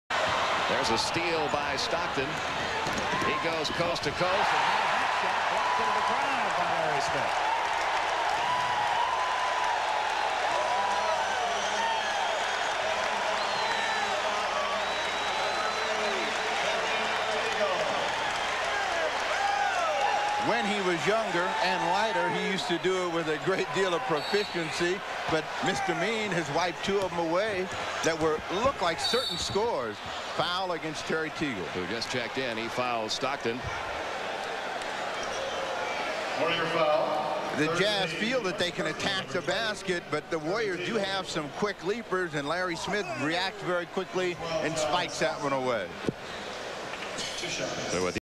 Larry "Mr. Mean" Smith Rejects Stockton
Mr. Mean swats away John Stockton's layup. May 2, 1989. Western Conference First Round, Game 3.